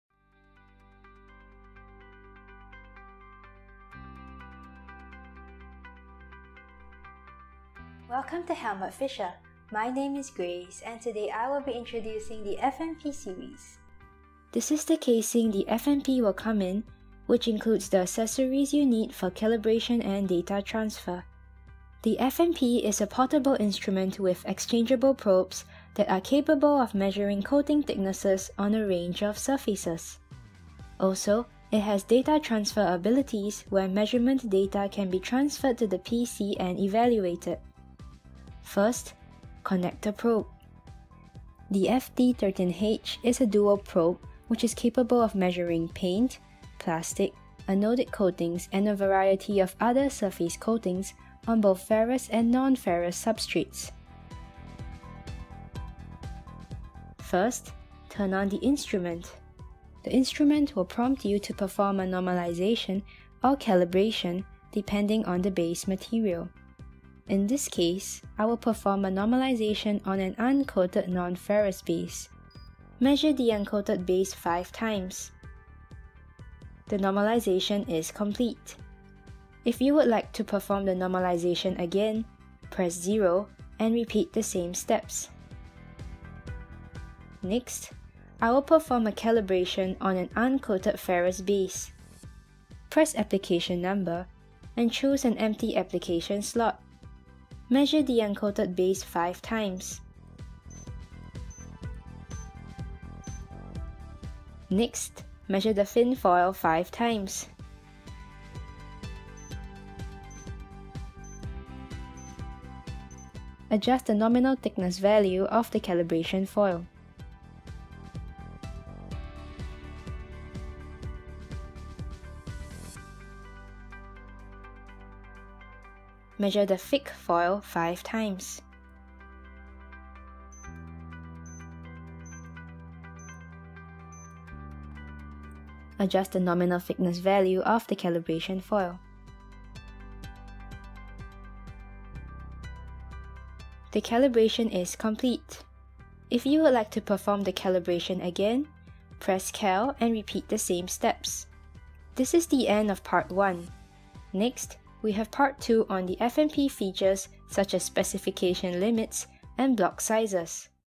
FMP Series Tutorial Part 1: Introduction | Gauges for Coating Thickness Measurement | Fischer
Tutorial Part 1: Introduction of the FMP series 🚩
The handheld coating thickness measurement devices from the FMP10 to FMP40 range deliver precise results and the various measurement techniques available make them highly flexible.

Models are available with magnetic inductive measurement (DELTASCOPE®), eddy current measurement (ISOSCOPE®) or both techniques combined (DUALSCOPE®).

By attaching different probes to these instruments, you quickly create the right solution for virtually any measurement task.